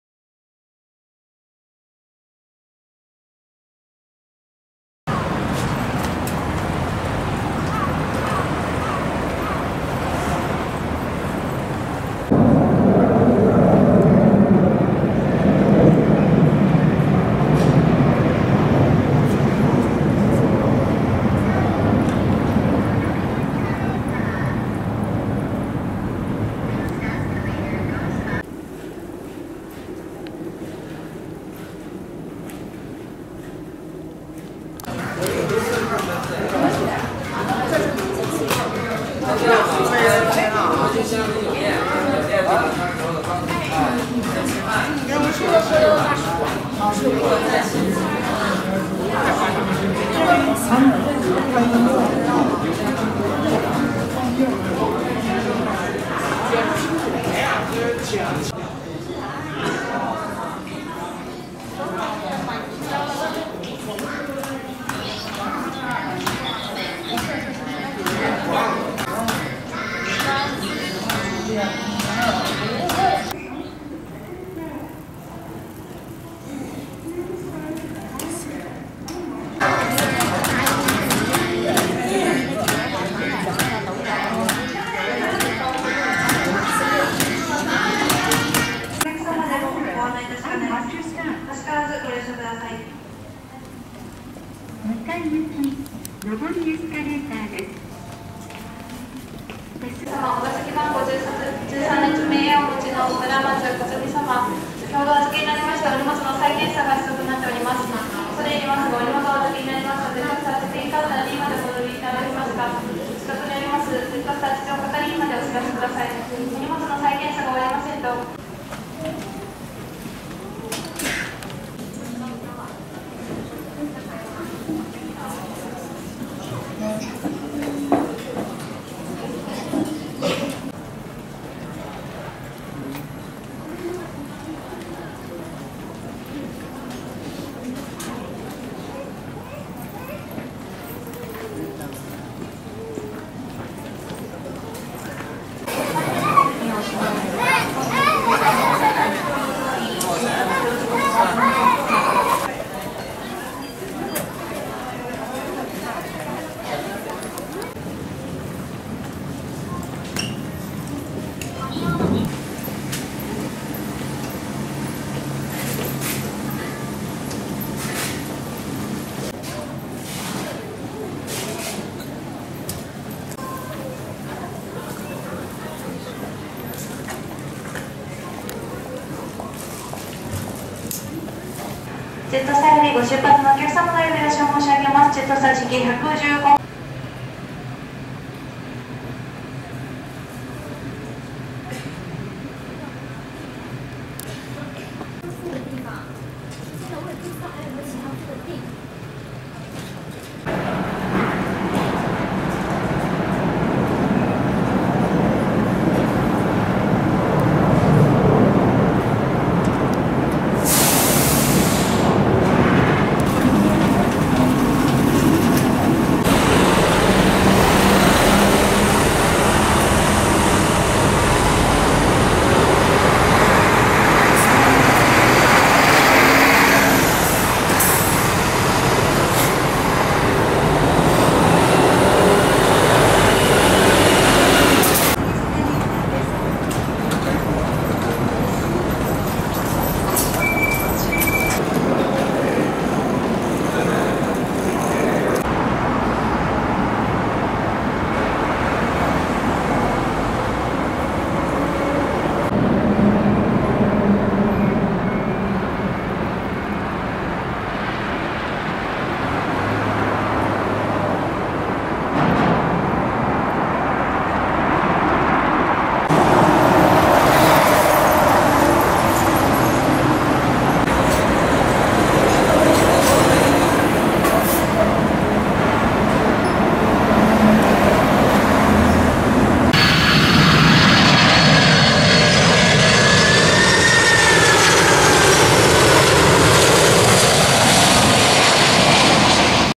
2019年9月に拡張された、LCC専用の【成田空港・第3ターミナル】/Terminal 3, Narita International Airport
2019年9月5日、成田国際空港の第3旅客ターミナルが拡張されました。今まで出発ロビーと到着ロビーは共に2階にあったのですが、到着ロビーを1階に移設しました。

千葉県成田市
Narita, Chiba Prefecture, Japan

◆YouTube(これが、成田空港・LCC(格安航空会社)専用の【第3ターミナル】(2019年8月))
◆YouTube(今はこんな感じです【成田空港・第3旅客ターミナル】(2020年3月15日))